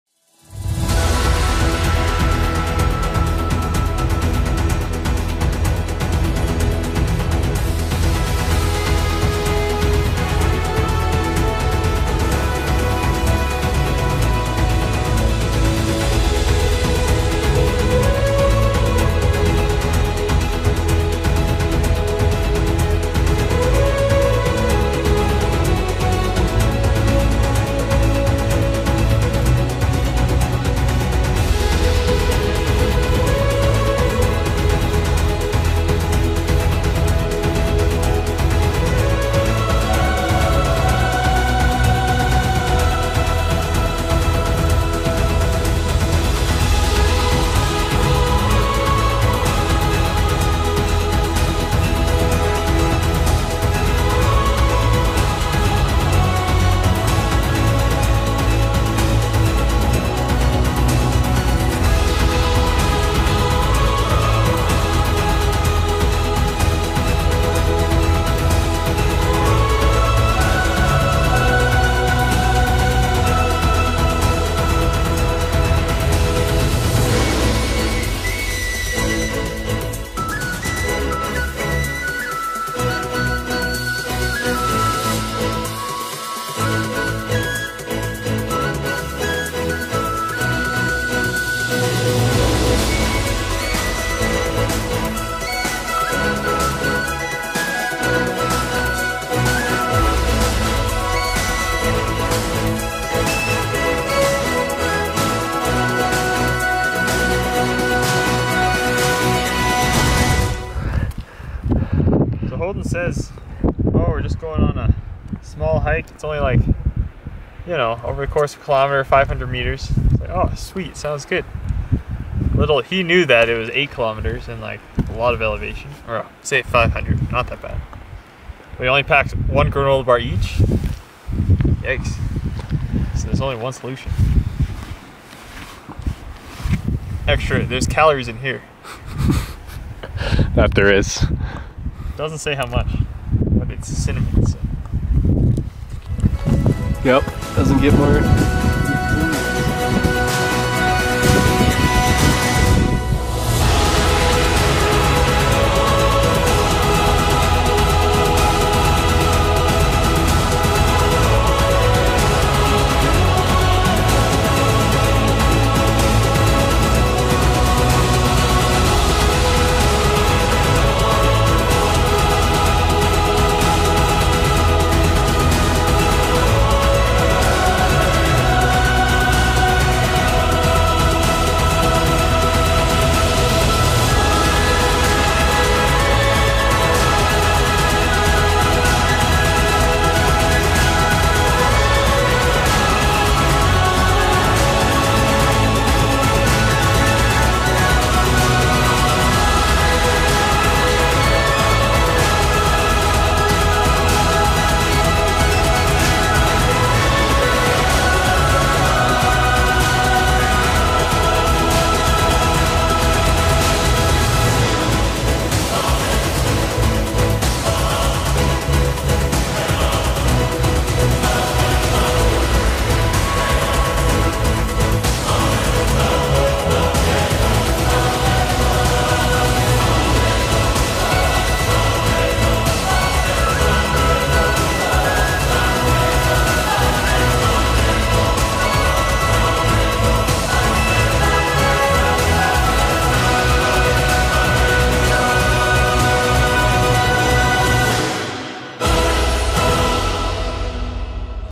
Lake O'Hara Alpine Hike
Day hike at Lake O'Hara in Yoho National Park, looping through Huber Ledges to Wiwaxy Pass, Oesa Lake, Yukness Ledges, and Opabin Lake.  July 29, 2019.

Filmed using an a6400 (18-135mm) on a Glidecam HD-1000